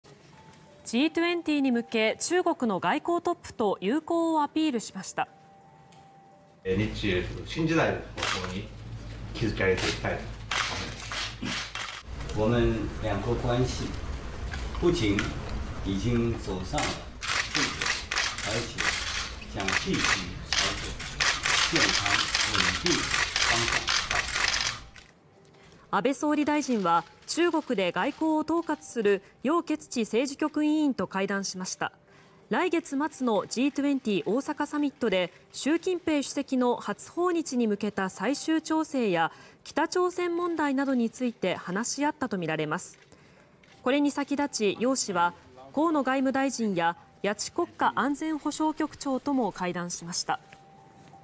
安倍総理と中国の外交統括 習主席の初来日調整か(19/05/17)
G20に向け、中国の外交トップと友好をアピールしました。